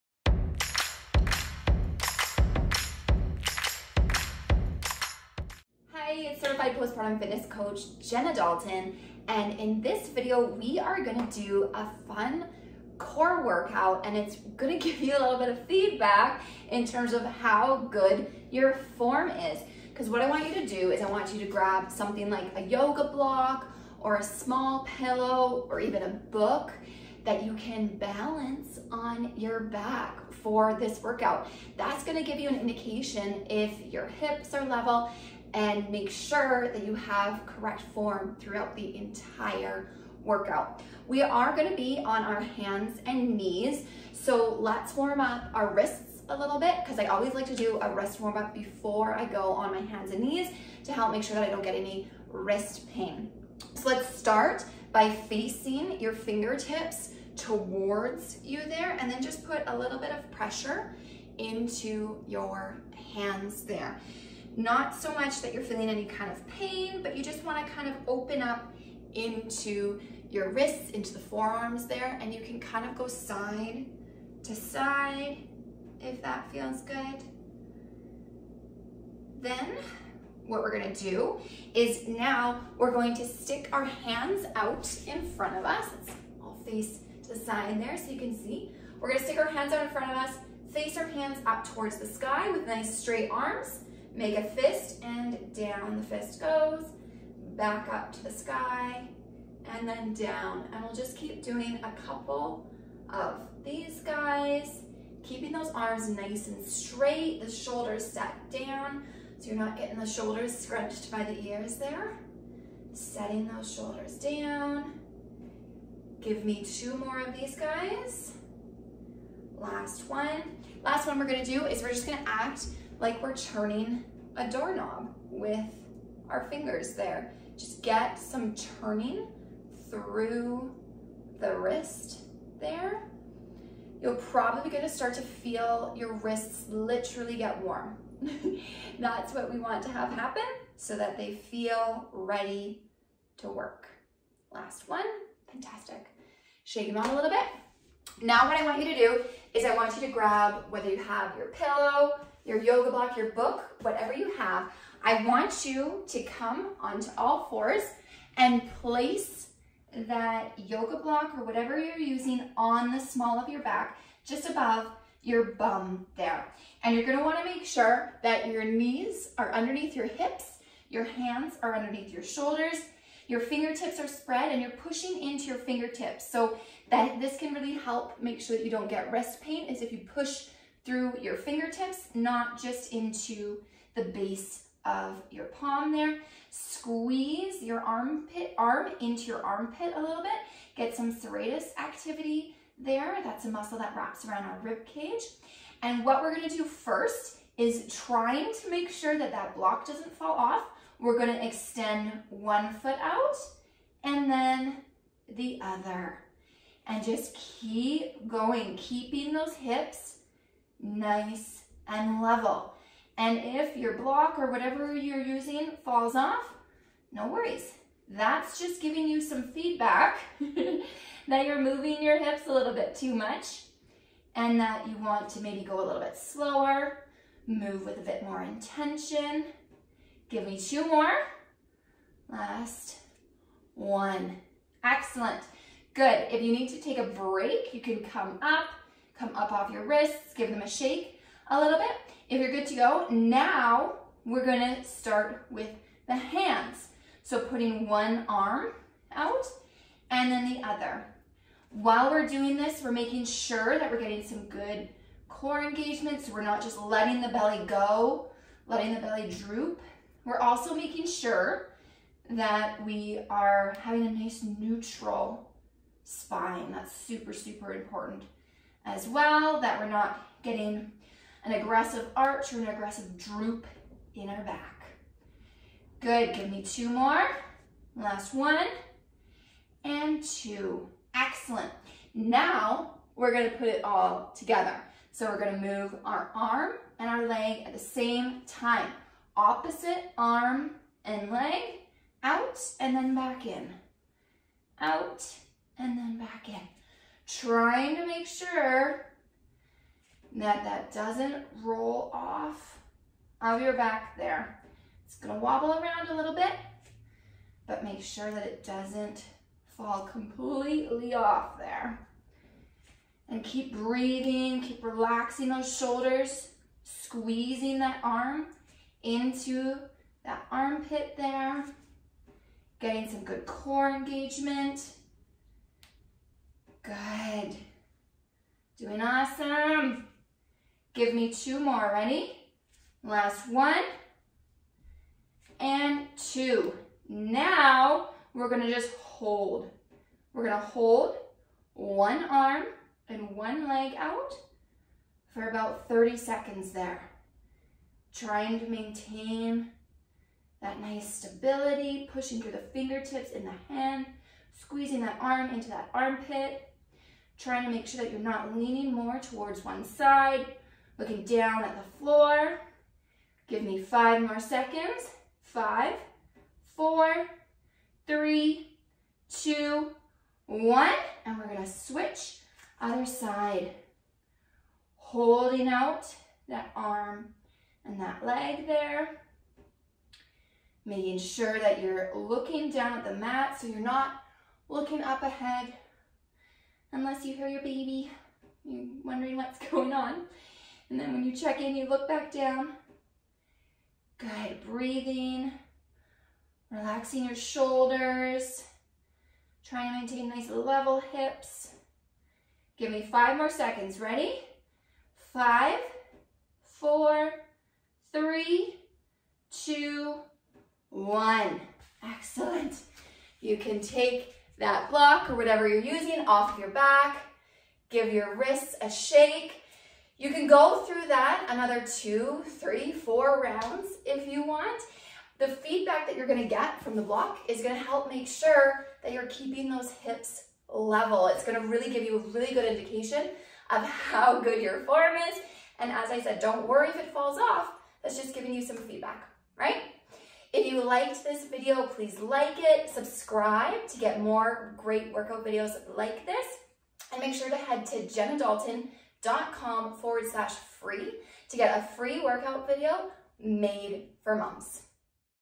Have You Tried This Core Challenge?
Try this core challenge to get stronger abs postpartum.